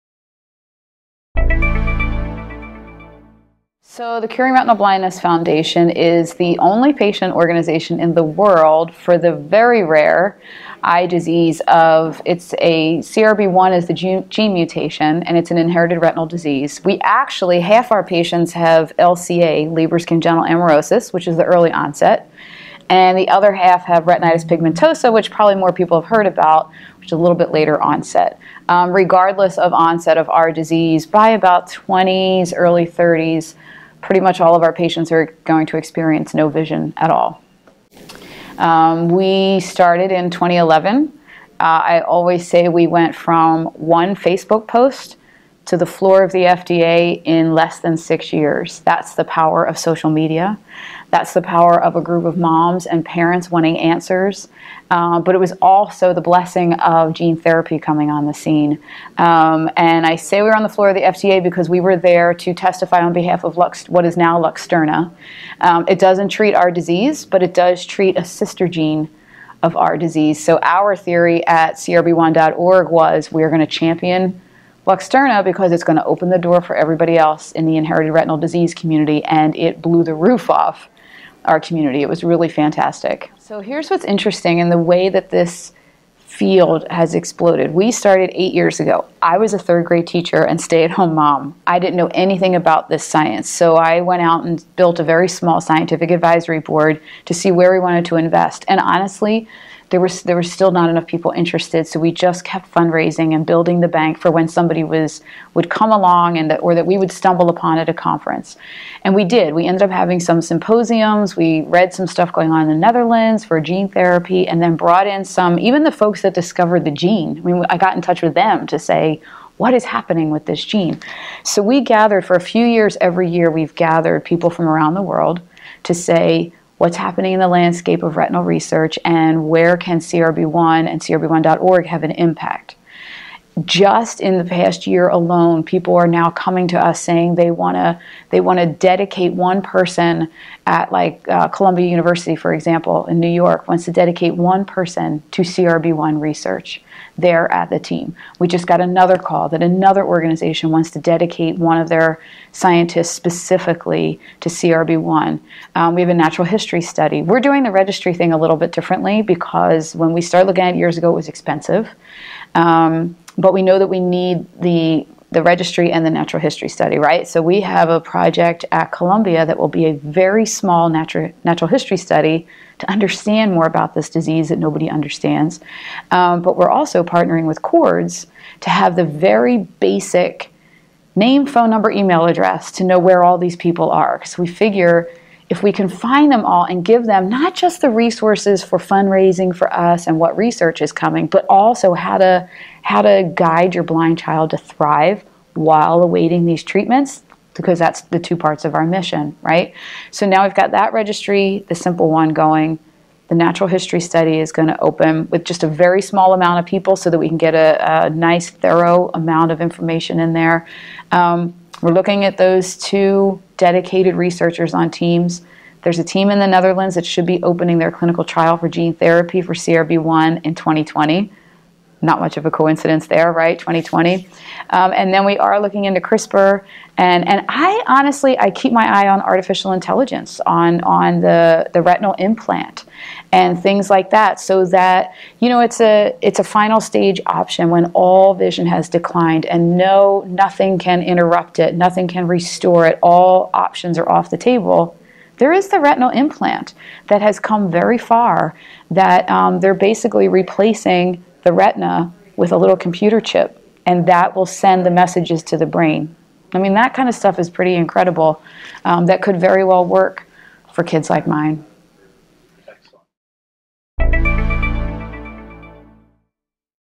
Retinal Blindness, Gene Therapy, CRISPR, and Artificial Intelligence
Kristin Smedley is the president of Curing Retinal Blindness Foundation, a non-profit organization focused on finding treatments and cures for CRB1 retinal disease.

Mutation in the CRB1 gene can lead to two rare conditions – retinitis pigmentosa and Leber congenital amaurosis.

Retinitis pigmentosa is the result of the photoreceptor cells of the eye – rods and cones – deteriorating. It is a slow degenerative disease in which rods usually decline first and as such, one of the earlier symptoms is difficulty seeing in dim light or transitioning from light to dark and dark to light. The disease is progressive and will eventually lead to total blindness when the person reaches their teens or early adulthood.

Leber congenital amaurosis is a more aggressively progressive disease that often appears at birth or soon after birth. Symptoms and severity can vary greatly among patients, but the gene mutations will cause loss of the photoreceptor cells and will lead to blindness.

In this video clip, Smedley discusses many of the studies underway in genetic retinal diseases, including the use of gene therapy, CRISPR, and artificial intelligence to possible treat these conditions.